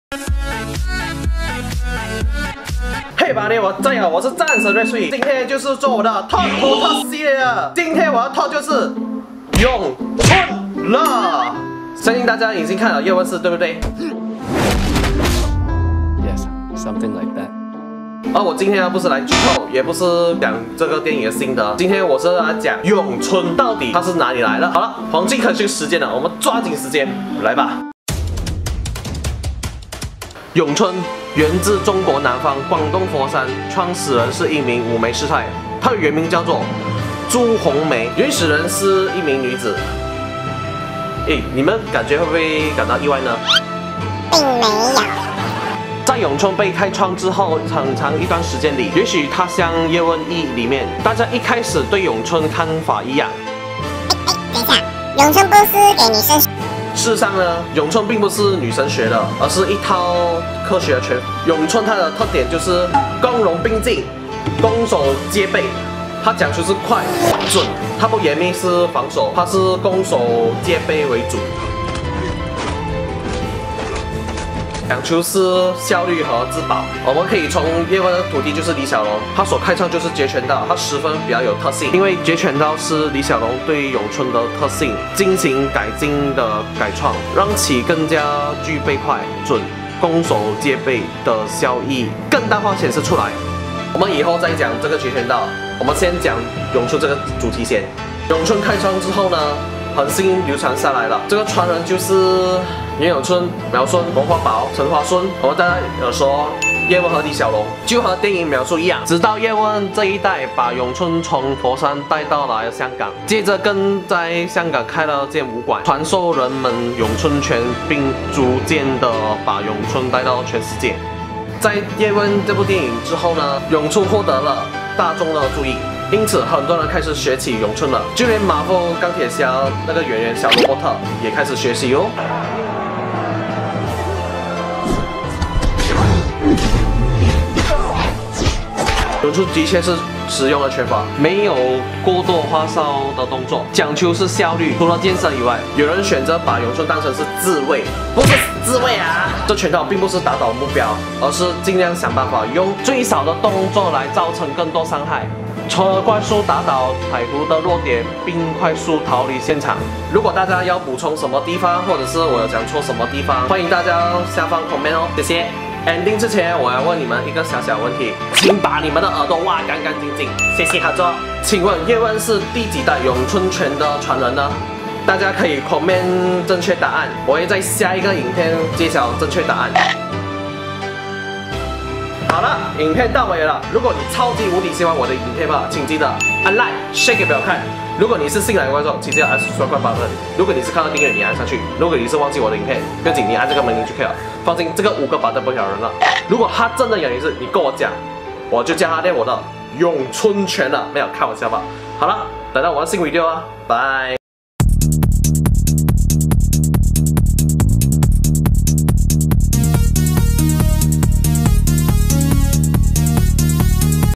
咏春拳来源【talk 2 talk #03】
这是讲解咏春拳的来源，这个咏春拳很久开创了，所以我们要要了解中国武术的历史和文化

使用相机
Canon EOS700D, HUAWEI nova 2 lite, HUAWEI nova 3i

其他器材
脚架=fotopro ufo2, wt3520
灯光=studio ring light 18inch
lens = EF-是18-55mm